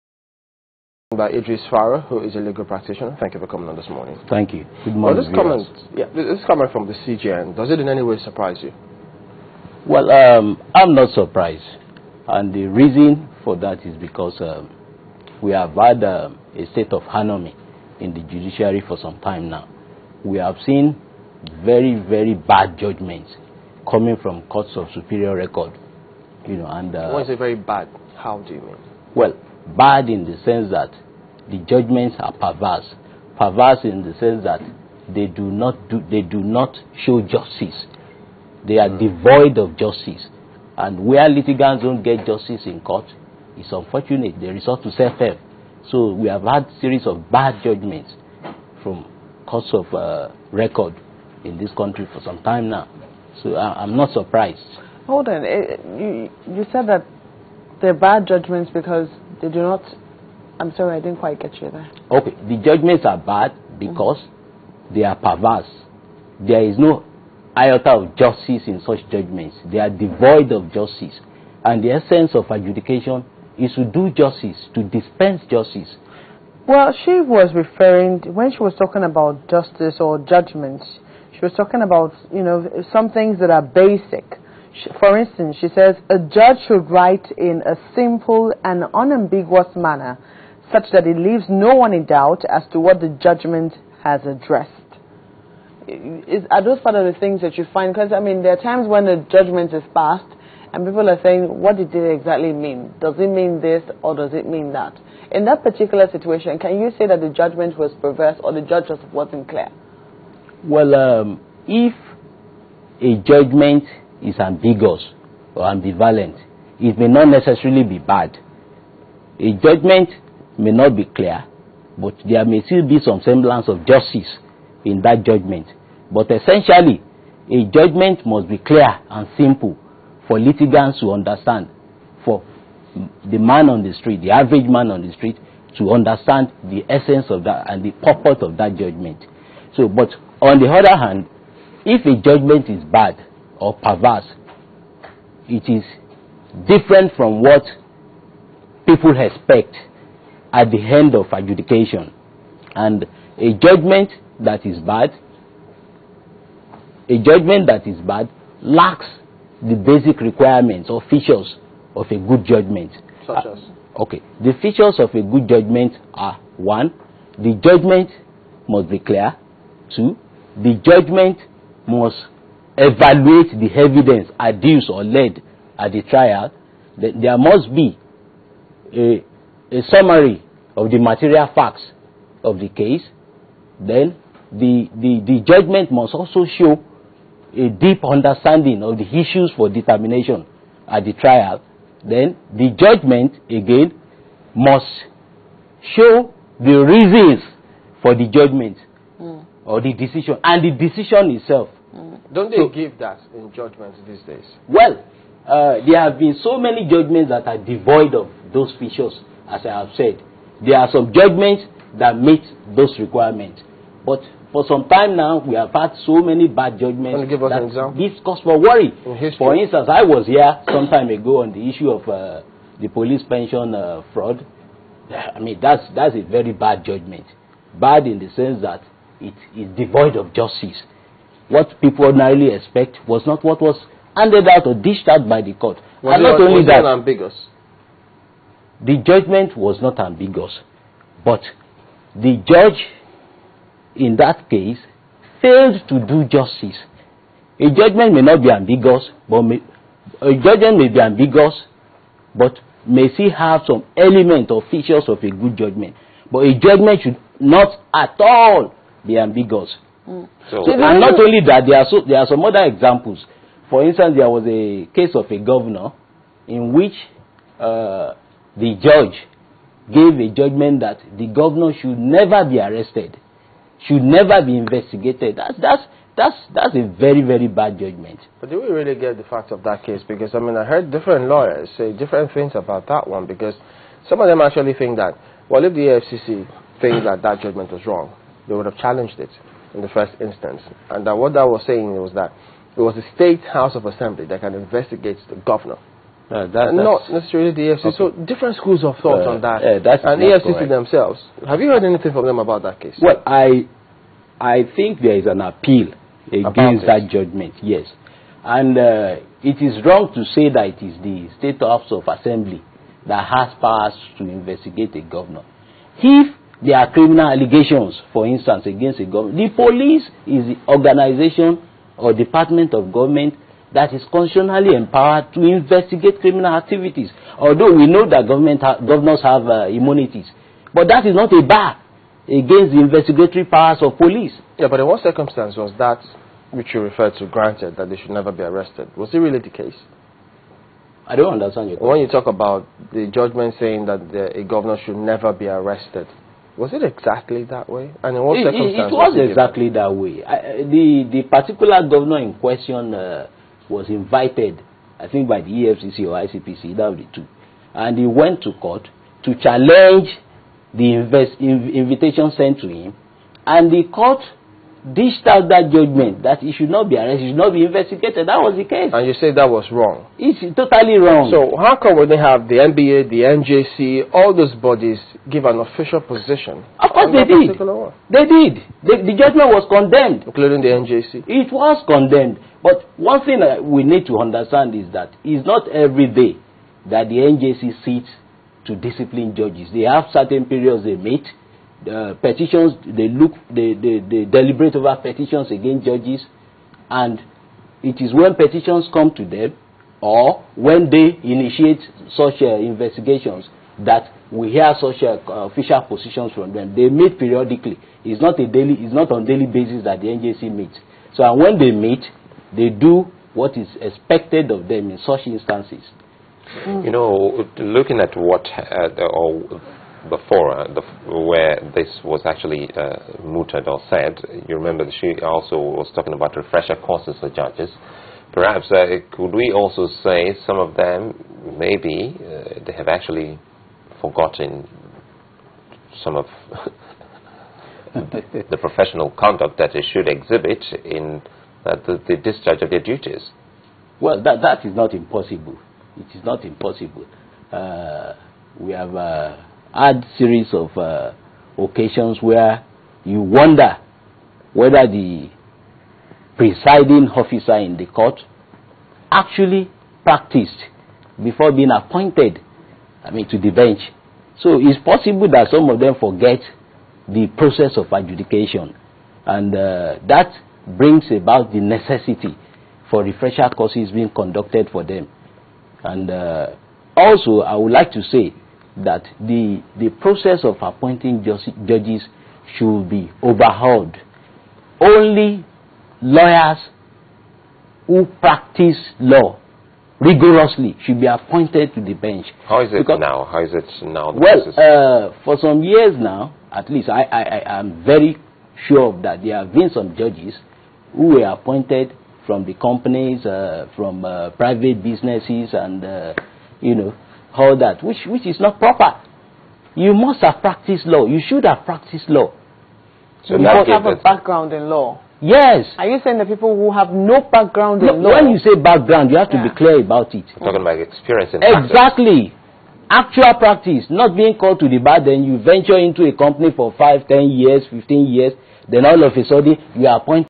Lawyer Recommends Ways To Restore Confidence In Judiciary - Part 1
A lawyer, Idris Faro has recommended refresher courses and training for judges in Nigeria as a means of curbing the decline the incidences of miscarried judgement.
Speaking as a guest on Channels Television's breakfast programme, Sunrise Daily, Mr Faro corroborated the assertions of the Chief Judge of Nigeria, (CJN), Justice Aloma Mukhtar that the performance of judges in delivering judgement is declining rapidly.
Speaking at a refresher course on how to write judgments organised for judges and Khadis by the National Judicial Institute (NJI) on Monday, the CJN said public confidence in justice delivery was beginning to nosedive.
She said: "Let us not deceive ourselves, the public confidence and trust in our work as judicial officers is beginning to nosedive.
"This is partly borne out from judgment emanating from our courts which the public or some section of it feels is rather unconscionable."
Reacting to the CJN's comment, Mr Faro said: "We have had series of occasion when one wonders whether the presiding officer in the court actually practiced before being appointed to the bench. It is possible that some of them forget the process of adjudication."
These according to the lawyer necessitate the need for refresher courses for the judges and for the overhauling of the process of appointment of judges.
He said it is improper to appoint lawyers in companies and private businesses as judges.
"Only lawyers, who practice law should be appointed to the bench," Mr Faro said.